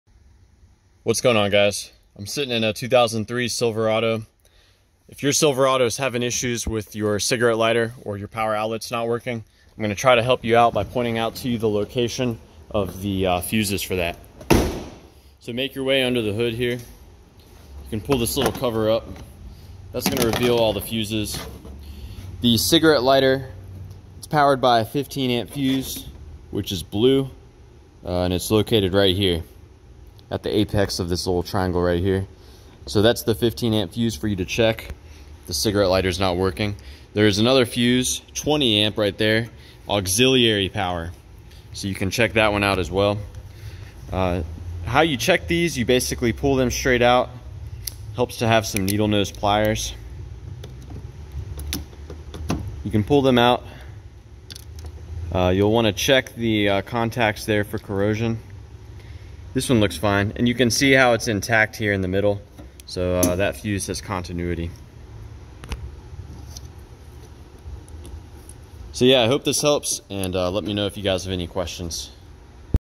2003 Silverado Cigarette Lighter Fuse Power Outlet Fuse
Using this 2003 Chevy Silverado 1500, I show you the location of the cigarette lighter fuse aka the power outlet fuse aka the aux power fuse.